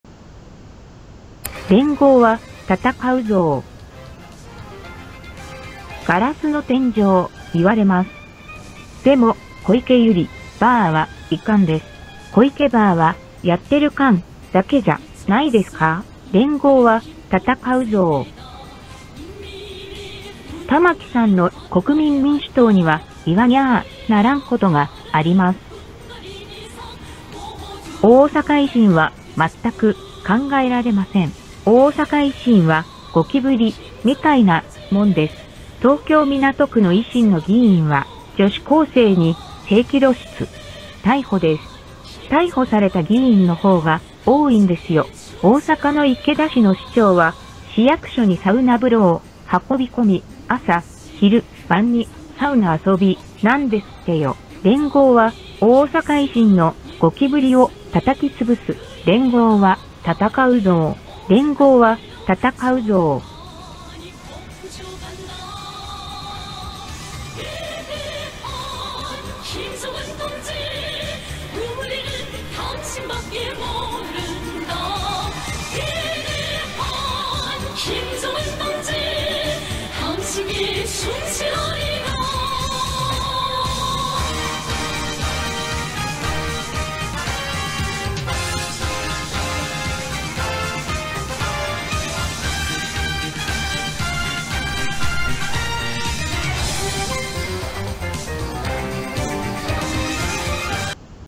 芳野友子連合会長、大阪維新に駄目だし。玉木氏に「真面目に生きよと、言ってやれ」と。
止まらない、大阪維新のゴキブリ。